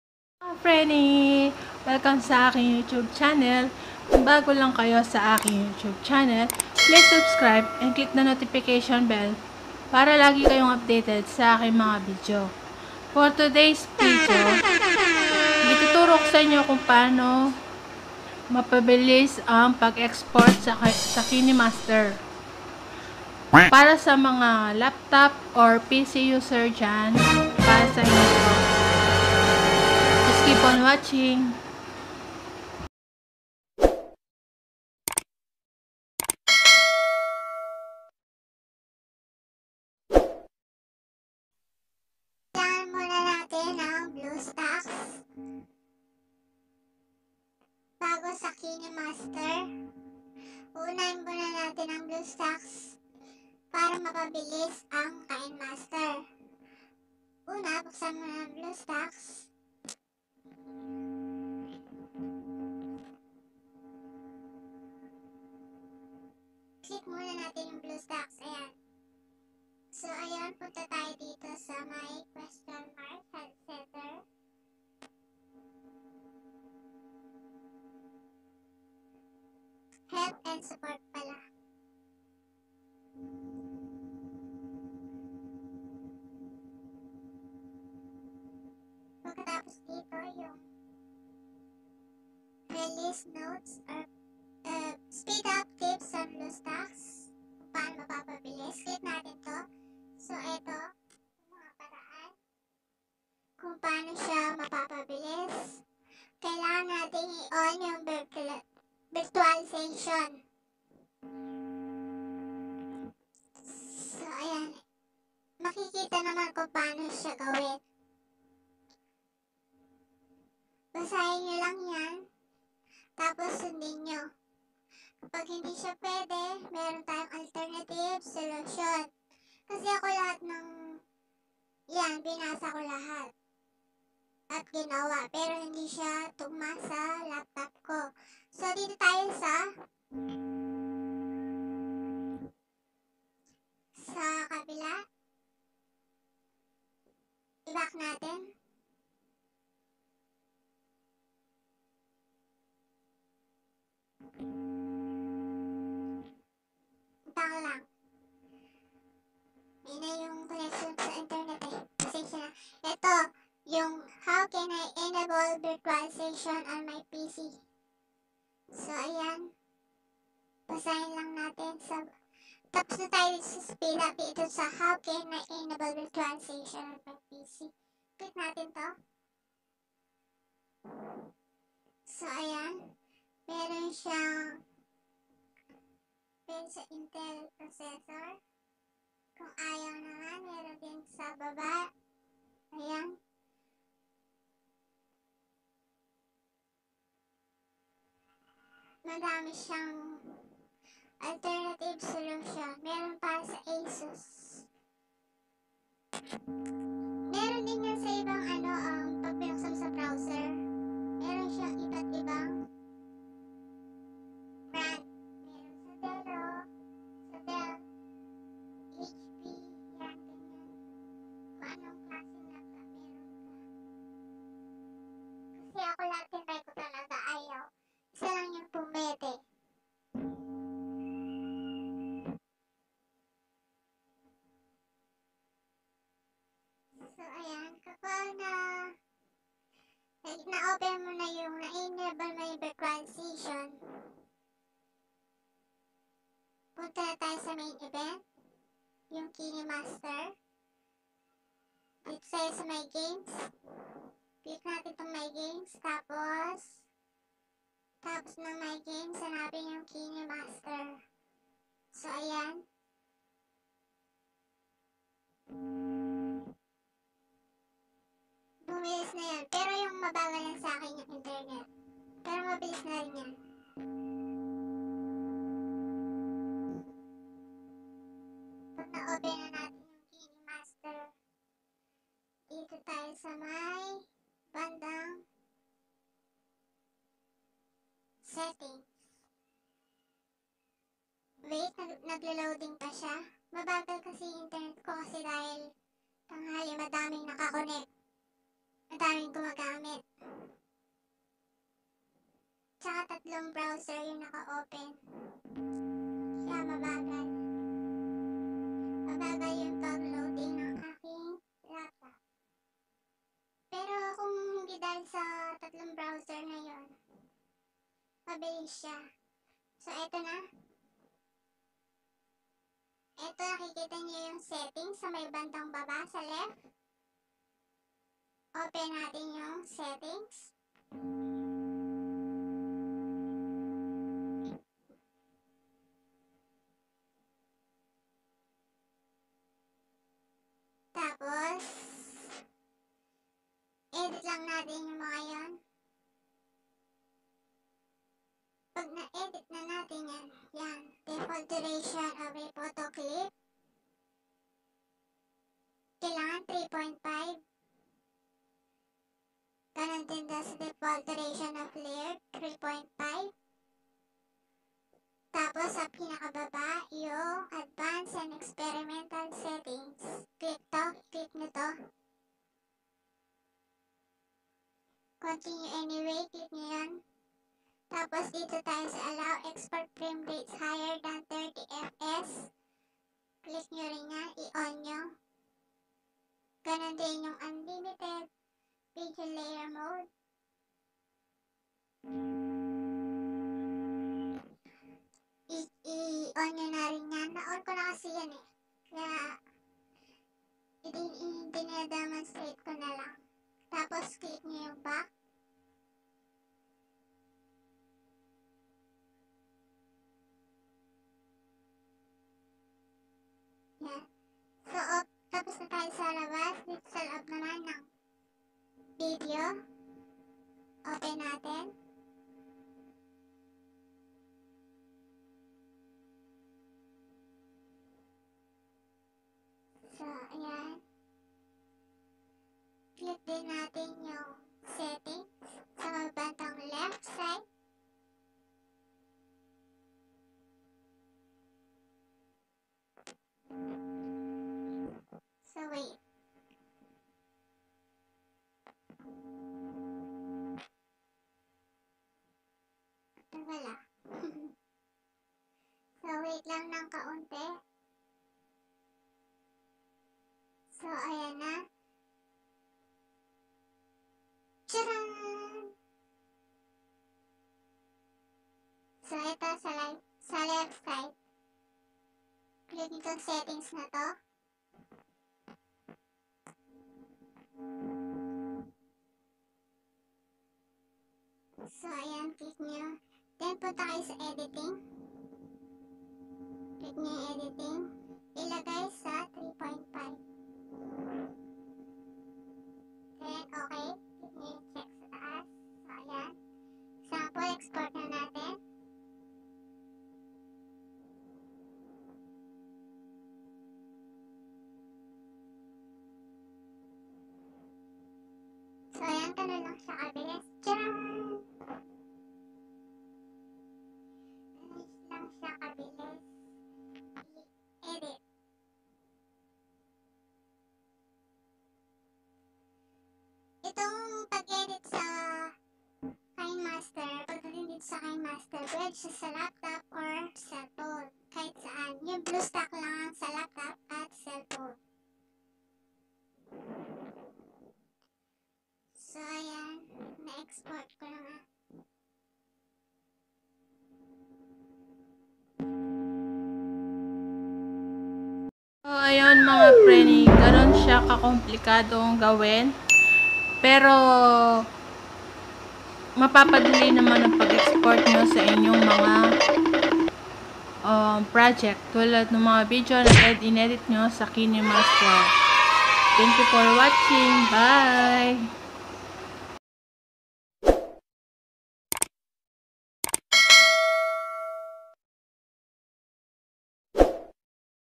HOW TO EXPORT FAST IN KINEMASTER?
para sa laptop and pc ang bluestacks kailangan muna ma enable vitualization bago mo iedit ung settings ng KINEMASTER. nakalimutan ko din idagdag sa video ko na kailangan iset sa